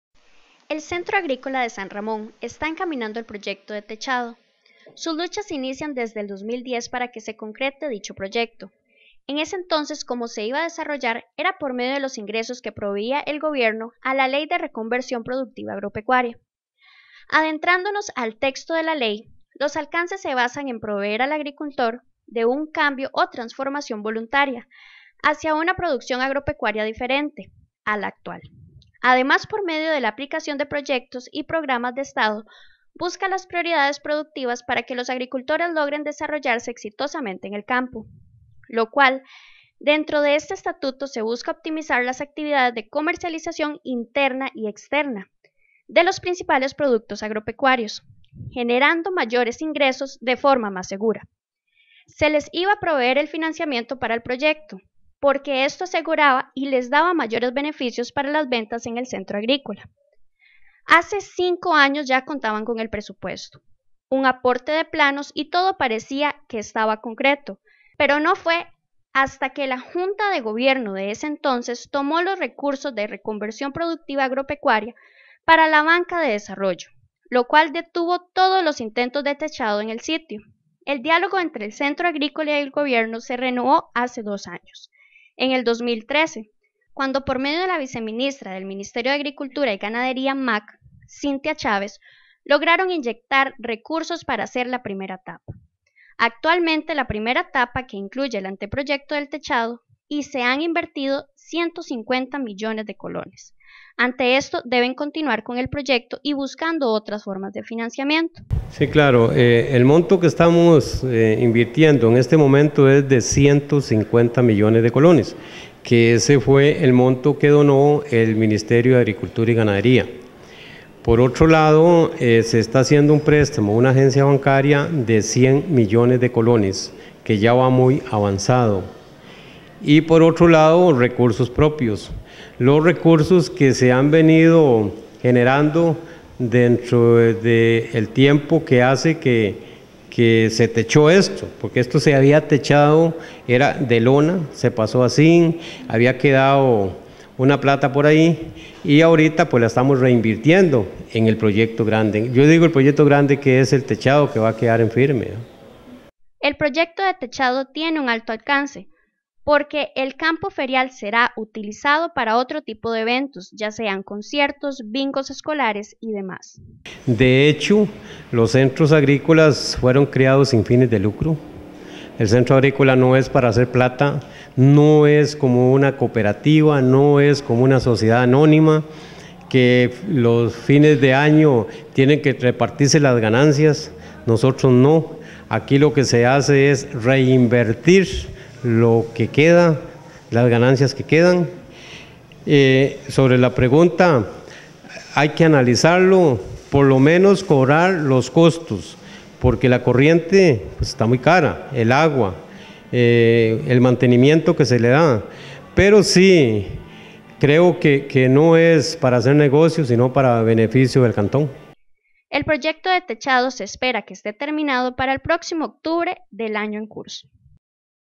Centro Agrícola Cantonal estrenará techado
Feria de Agricultura Ramonense está tramitando distintos recursos para terminar con el proyecto de techado, este está previsto que será terminado en octubre del año en curso.